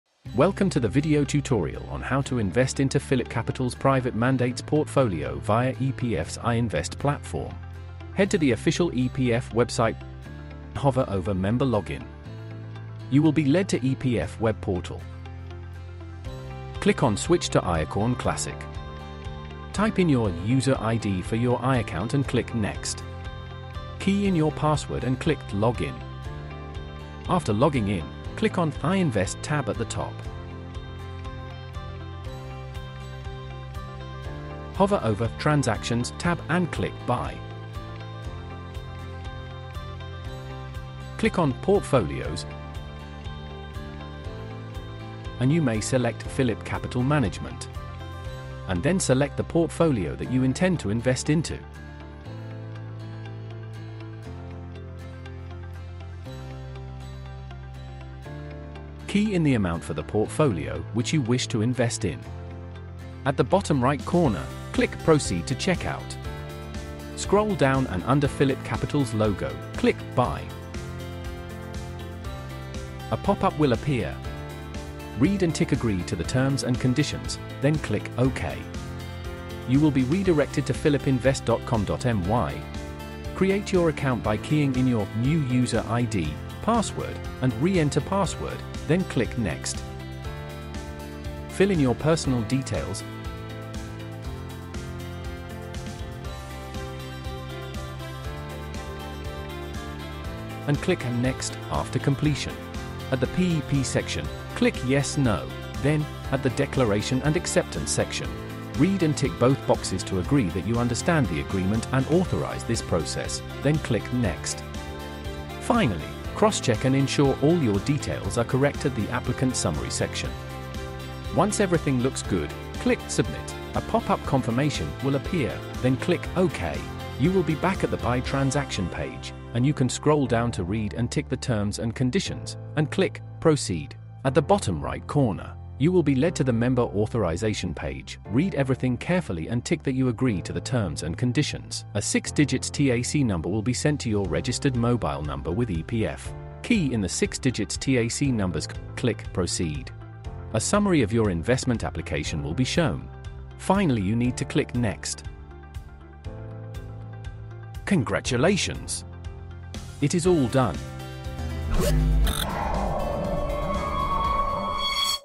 Simple Step-by-Step Tutorial You Need To Know: Buy Private Mandate Portfolios on I-Invest Platform
Phillip Capital Management Sdn Bhd (PCM) proudly leads as the FIRST Fund Management Institution (FMI) under the Members Investment Scheme to introduce Phillip Managed Account for Retirement (PMART) on the i-Invest platform.

Starting from 24th January 2024, EPF has announced that its members can now invest, switch, or redeem their Private Mandate Portfolios through the secure and user-friendly online transaction portal (i-Invest) within the i-Akaun (member) portal.

Through i-Invest (i-Akaun), EPF members can also effortlessly view their Private Mandate Portfolios and select the PMART products they wish to invest.

Start your investment journey with us today from RM5,000*!